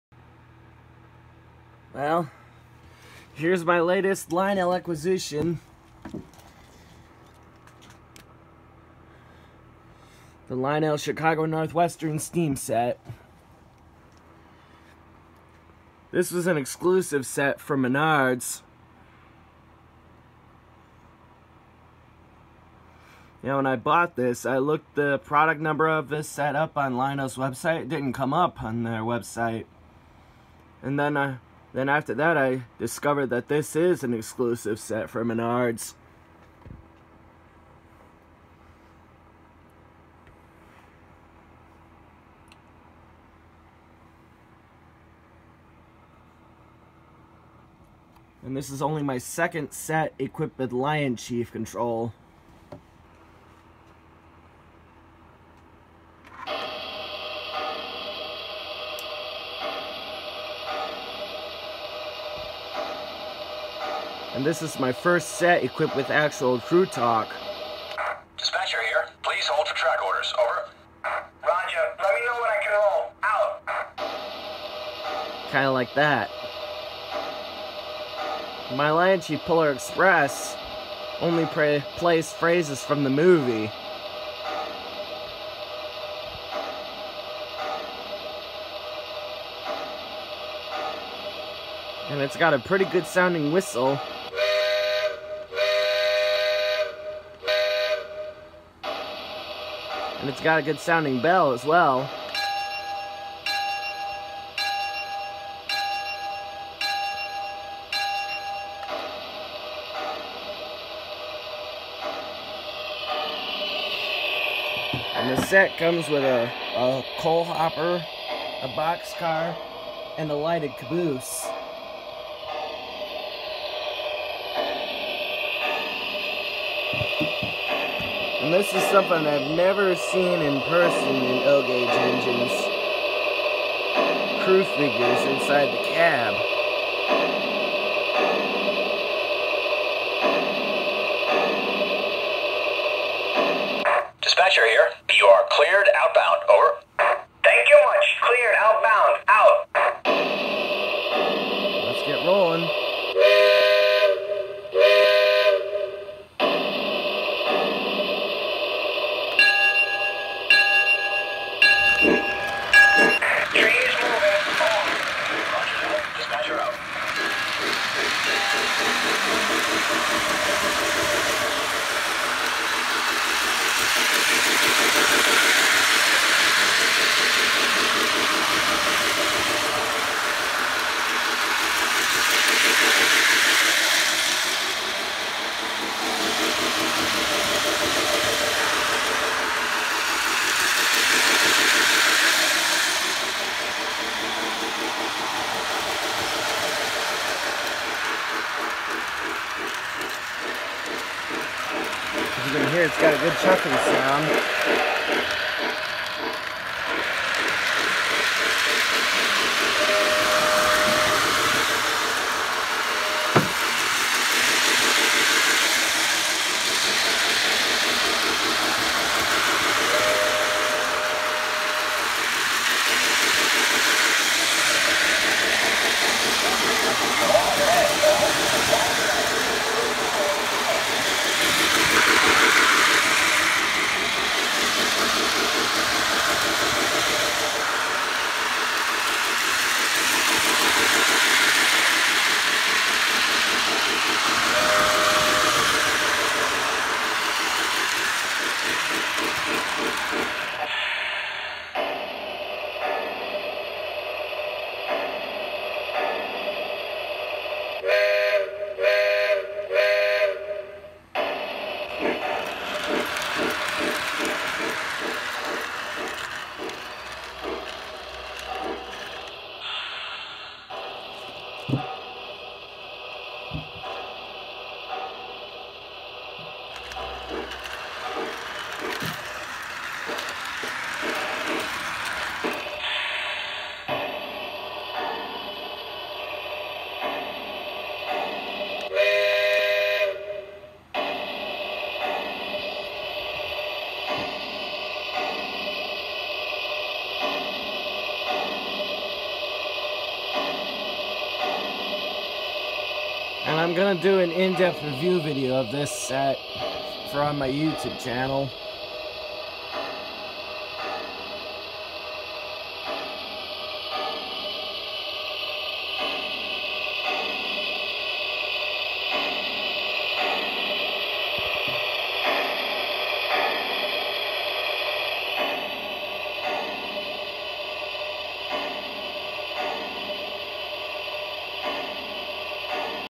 Lionel LionChief Chicago & Northwestern Steam Set
Here’s another preview of my new Lionel Chicago & Northwestern steam set with LionChief control. This set was made exclusively for Menard’s. The reason I know this is because I looked up the product number of this set on Lionel’s website and it didn’t show up.